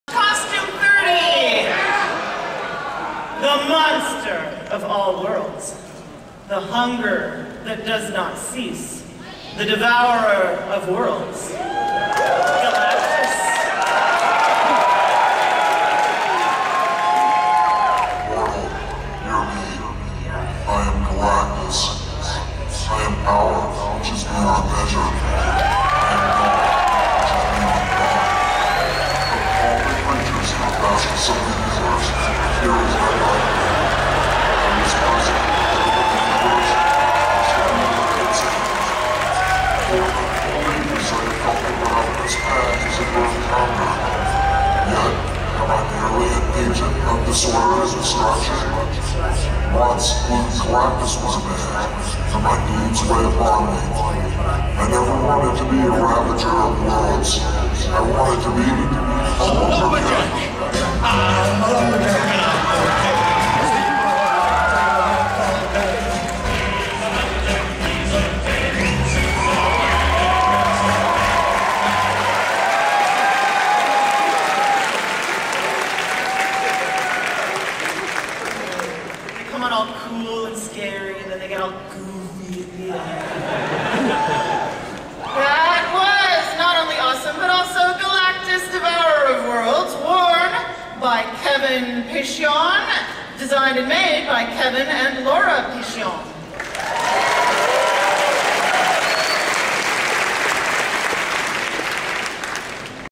San Diego Comic Con 2012 Masquerade - #29 Galactus Devourer of Worlds
Video of Entry 29, presentation of Galactus Devourer of Worlds Marvel Comics performed at San Diego Comic Con 2012 Masquerade .  Filming credits to Mario Bueno (Permission granted)
(Won Honorable Mention for Re-Creation, David C. Copley Prize)